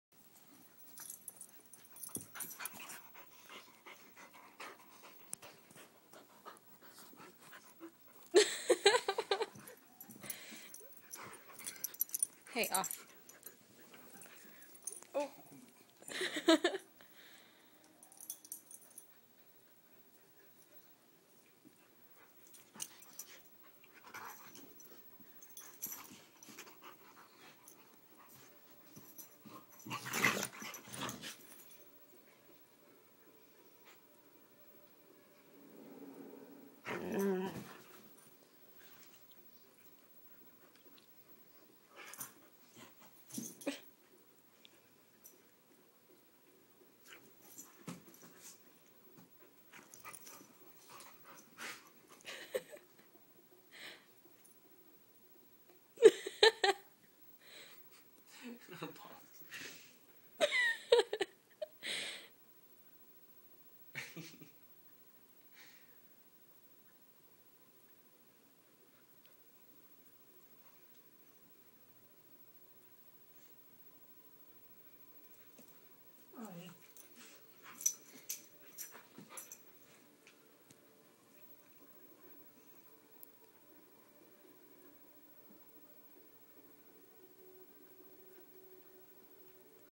You play, we play!
If there is even one person paying attention to something other than these two, they start playing right in the middle of whatever is going on. Churchill at 16 weeks, Sofia at 6 years.